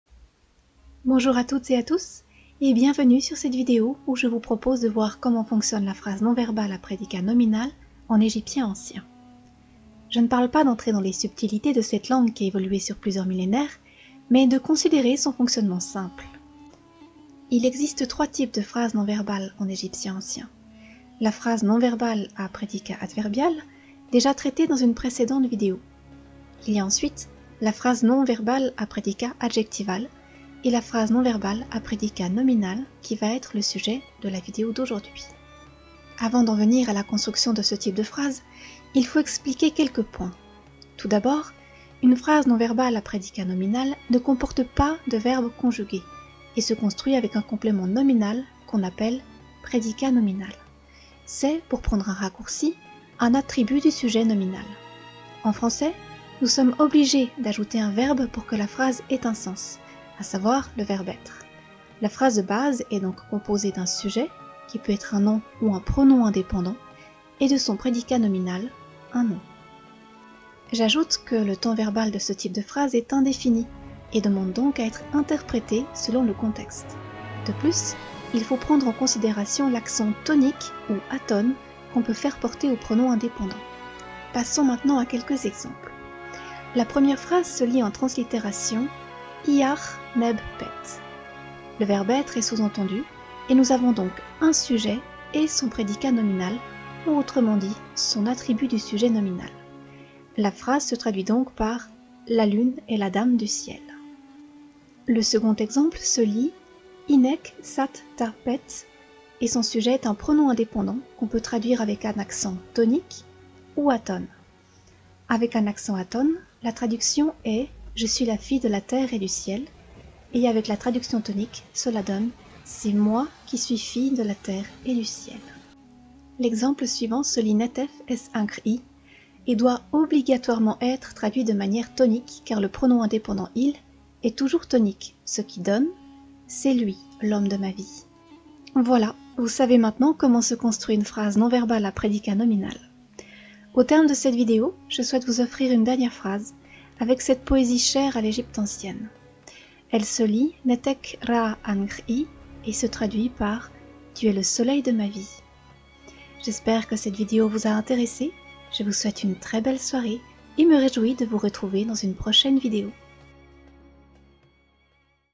Lire et écrire les hiéroglyphes égyptiens #4 : la phrase non verbale à prédicat nominal
Construction d'une phrase non verbale à prédicat nominal simple en égyptien ancien.
CETTE VIDÉO EST ADAPTÉE À TOUS LES PUBLICS.

Musiques et images libres de droit.

Ouvrages utiles :
FISCHER H. G., Ancien Egyptian Calligraphy : A beginner’s guide to written hieroglyphs, New York : 1999 (4e édition).
GARDINER A. H., Egyptian Grammar : Being an Introduction to the Study of Hieroglyphs, OXFORD : 1957 (3e édition).
BONNAMY Y., Dictionnaire des hiéroglyphes, Paris : 2010.